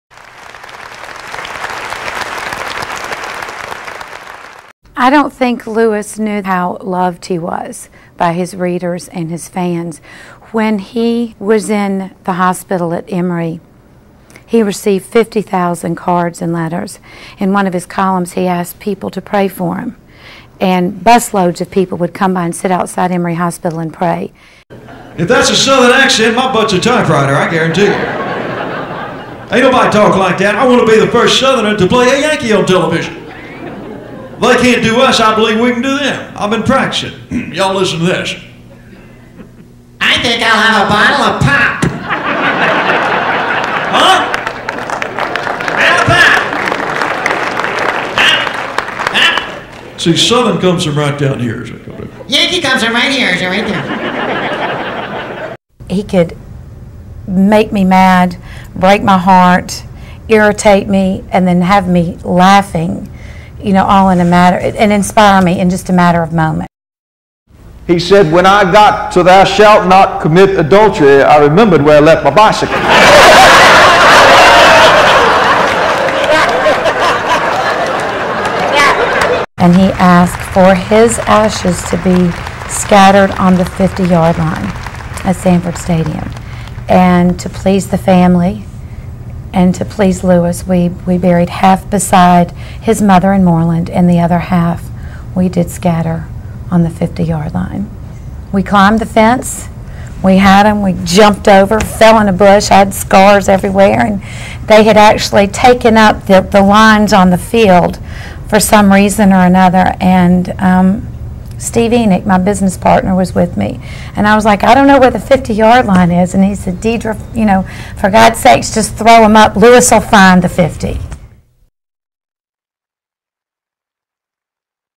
Lewis Grizzard: In His Own Words (comments from Dedra Grizzard)
Lewis Grizzard's widow, Dedra, talks about the late author and humorist. This is the 10th year of continuous performances of LEWIS GRIZZARD: IN HIS OWN WORDS, a tribute stage show with actor Bill Oberst Jr. in the role of Lewis. The show has played over 700 sold-out performances. Read reviews at the Official Lewis Grizzard